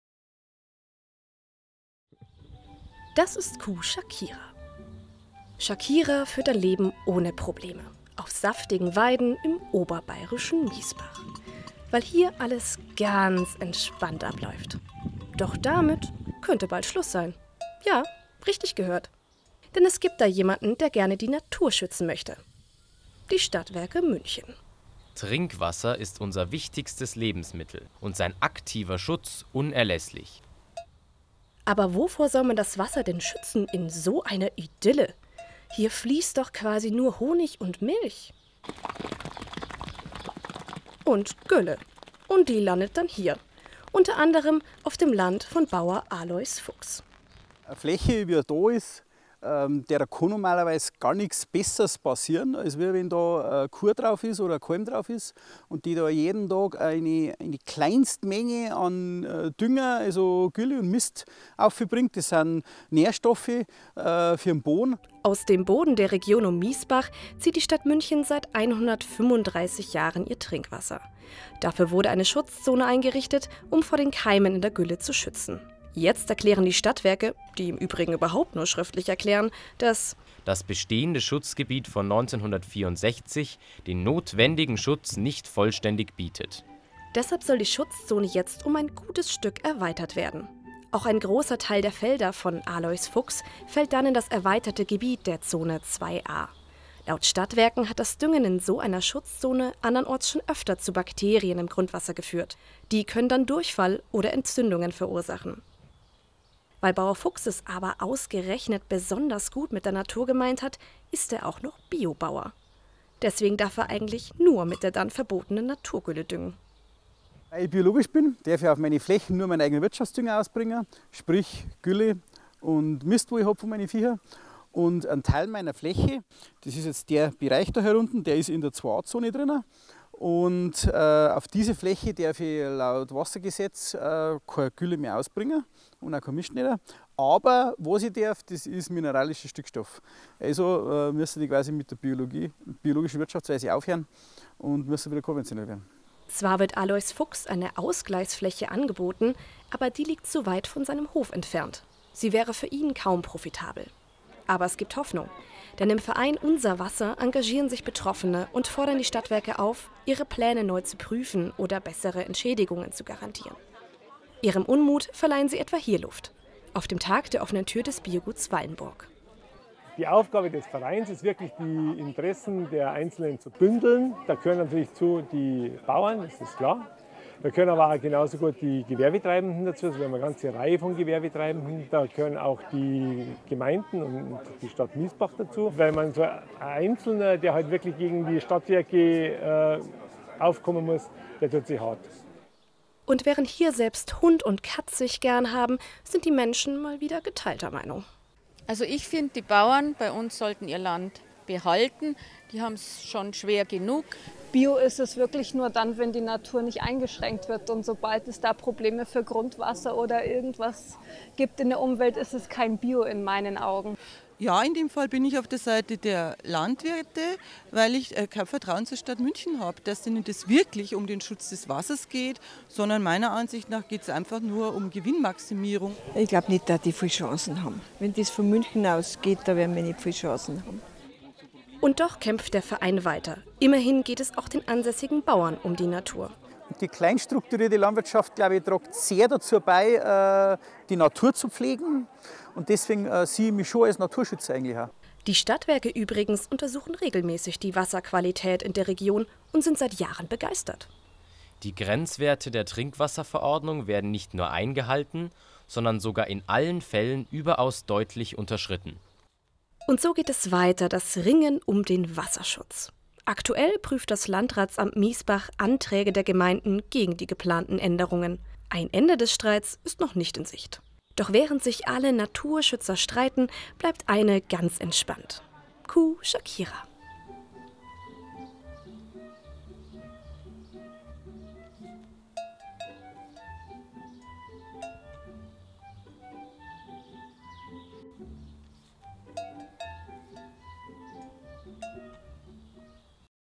Einsteins 2018 - Ein neues Klo für Kuh Shakira
Im oberbayerischen Miesbach soll das Wasserschutzgebiet der Stadt München erweitert werden. Einige der Landwirte vor Ort dürften deshalb einen Teil ihrer Flächen bald nicht mehr zur Beweidung nutzen, weil die Gülle ihrer Kühe laut Stadtwerken das Grundwasser gefährden könnte. Alois Fuchs ist Bio-Bauer und düngt entsprechend nur mit Gülle, um seine Fläche weiter bewirtschaften zu können müsste er wieder auf Chemie zurückgreifen. Im Verein “Unser Wasser e.V.” organisieren sich die Bauern um gegen die Erweiterung zu kämpfen, Aussichten auf Erfolg haben sie dabei aber kaum.